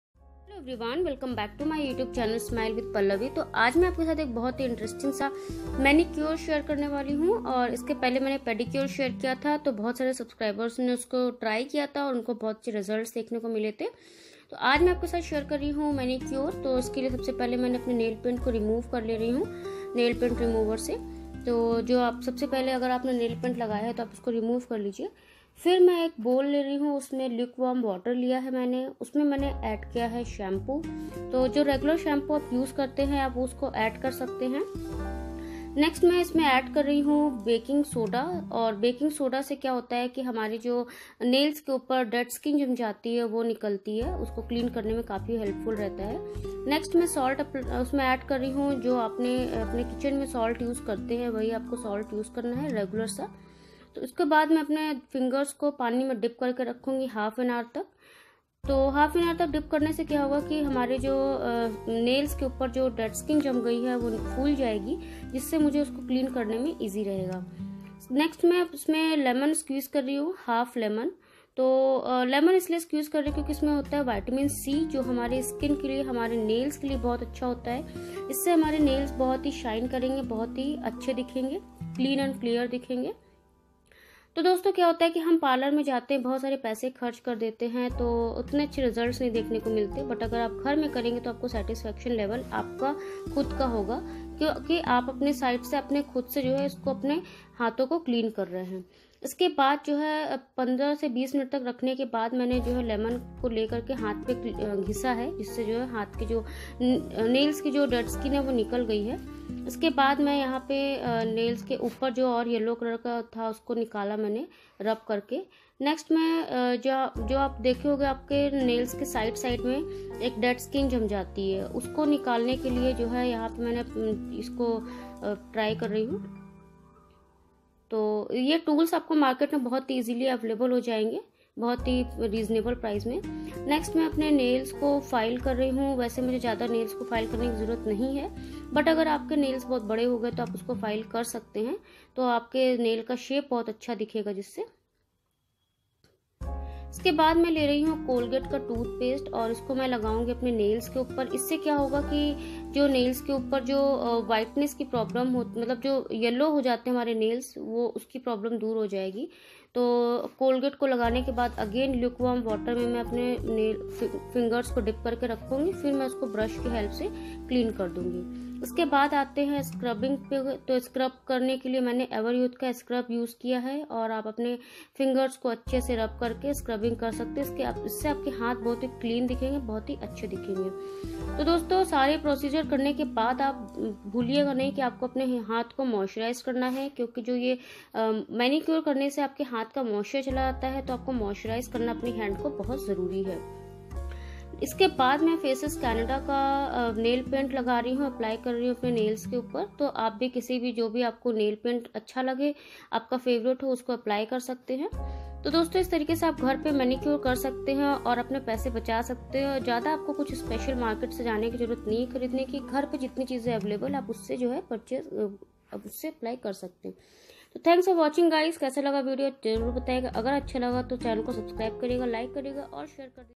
पार्लर जैसा Manicureघर पर करें | Salon Style Manicure At Home#howtodosalonstylemanicure#monsoonhacks
I am sharing how to do Manicure at home, How to do Parlour type manicure at home, You Must know this and you will see the changes in your hand beauty, So guys if you like this video then plz Subscribe the channel to get more informative videos.